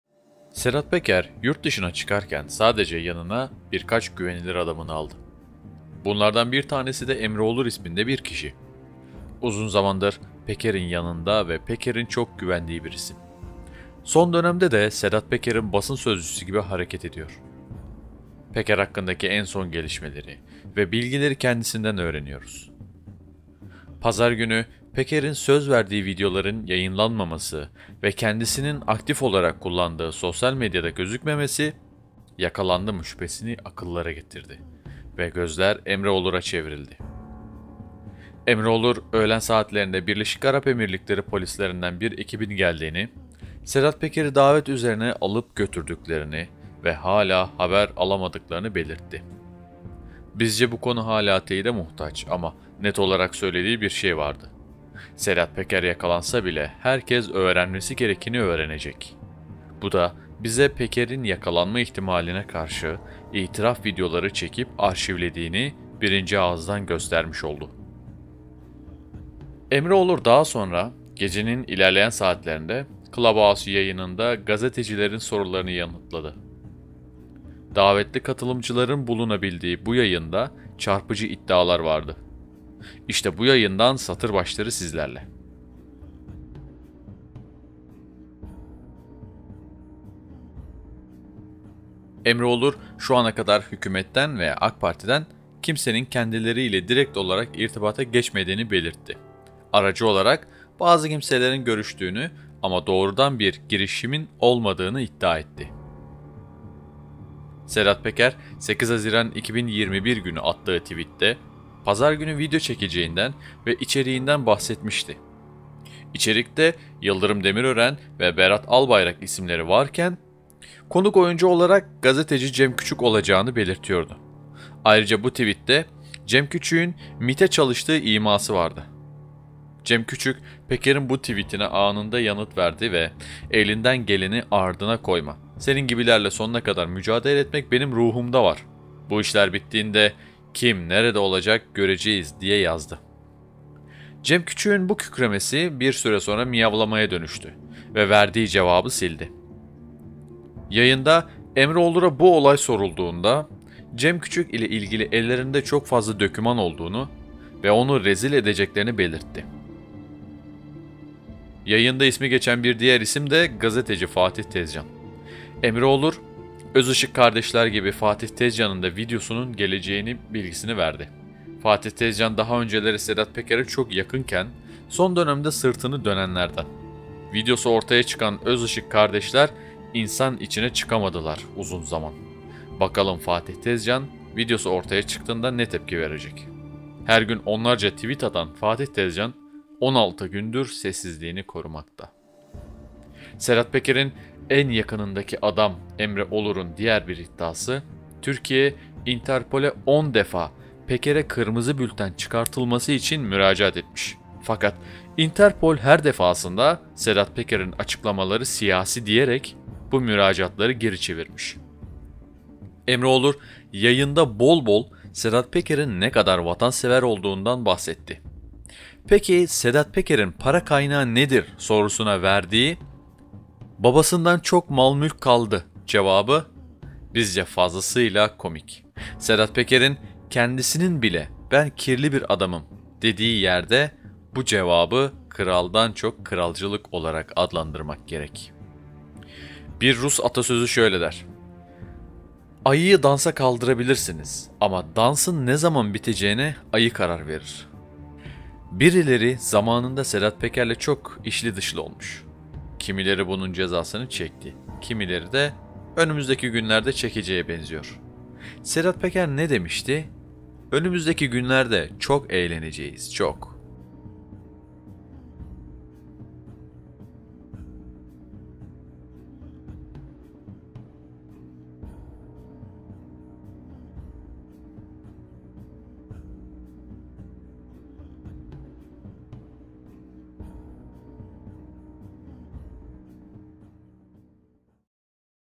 Sedat Peker'in en yakınındaki adamı Emre Olur bomba açıklamalar yaptı
Sedat Peker yurtdışına çıkarken sadece yanına birkaç güvenilir adamını aldı. Bunlardan bir tanesi de Emre Olur isminde bir kişi. Uzun zamandır Peker'in yanında ve Peker'in çok güvendiği bir isim. Son dönemde de Sedat Peker'in basın sözcüsü gibi hareket ediyor. Peker hakkındaki en son gelişmeleri ve bilgileri kendisinden öğreniyoruz. Pazar günü Peker'in söz verdiği videoların yayınlamaması  ve kendisinin, aktif olarak kullandığı sosyal medyada gözükmemesi,  yakalandı mı ? şüphesini akıllara getirdi ve gözler Emre Olura çevrildi. Emre olur, öğlen saatlerinde Birleşik Arap Emirlikleri polislerinden bir ekibin geldiğini , Sedat Peker'i davet üzerine alıp götürdüklerini ve hala haber alamadıklarını belirtti. Bizce bu konu hala teyide muhtaç ama net olarak söylediği bir şey vardı; “Sedat Peker yakalansa bile herkes öğrenmesi gerekeni öğrenecek.” Bu da bize Peker'in yakalanma ihtimaline karşı itiraf videoları çekip arşivlediğini birinci ağızdan göstermiş  oldu.
Emre Olur daha sonra gecenin ilerleyen saatlerinde Clubhouse yayınında gazetecilerin sorularını yanıtladı. Davetli katılımcıların bulunabildiği bu yayında çarpıcı iddialar vardı. İşte bu yayından satır başları sizlerle.